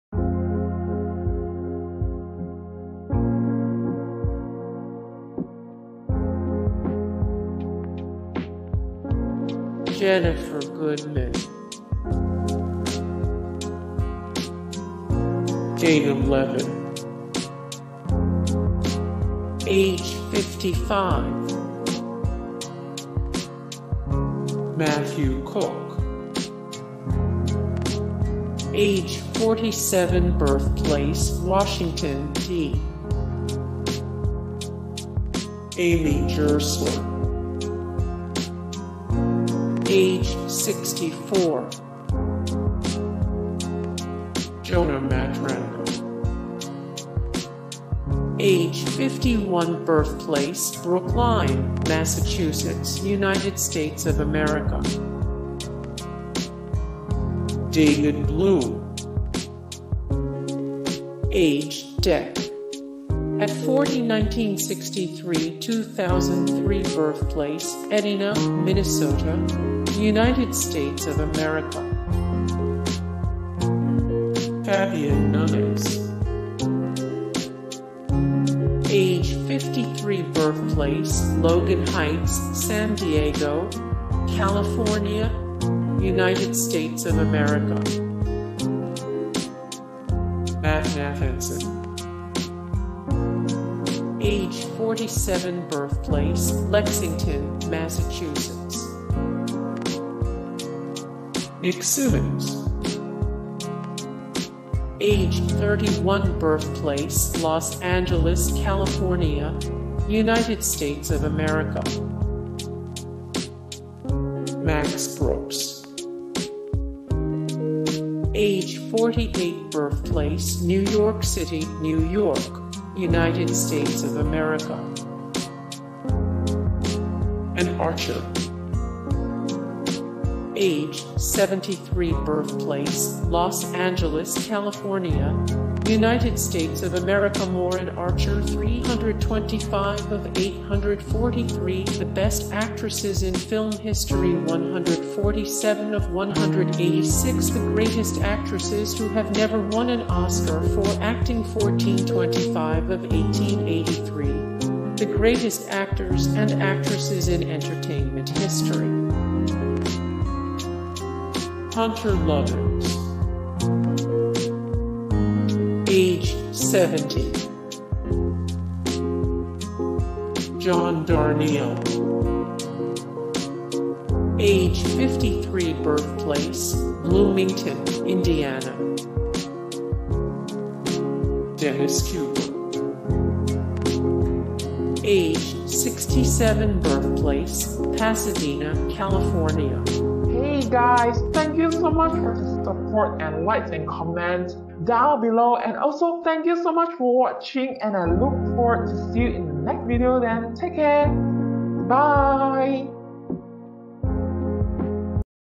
Famous Pitzer College Alumni ➊
List of famous alumni from Pitzer College, with photos when available. Prominent graduates from Pitzer College include celebrities, politicians, business people, athletes and more. This list of distinguished Pitzer College alumni is loosely ordered by relevance, so the most recognizable celebrities who attended Pitzer College are at the top of the list. This directory is not just composed of graduates of this school, as some of the famous people on this list didn't necessarily earn a degree from Pitzer College.  The list you're viewing is made up of a variety of different graduates, including Anne Archer and Dennis Cooper. This list answers the questions Which famous people went to Pitzer College? and Which celebrities are Pitzer College alumni?...more
0:00 - Intro
0:00:08 - Jenniphr Goodman
0:00:15 - Dana Levin
0:00:25 - Matthew Cooke
0:00:37 - Amy Gerstler
0:00:47 - Jonah Matranga
0:01:01 - David Bloom
0:01:19 - Fabian Nez
0:01:36 - Matt Nathanson
0:01:48 - Nick Simmons
0:02:02 - Max Brooks
0:02:16 - Anne Archer
0:02:51 - Hunter Lovins
0:03:01 - John Darnielle
0:03:13 - Dennis Cooper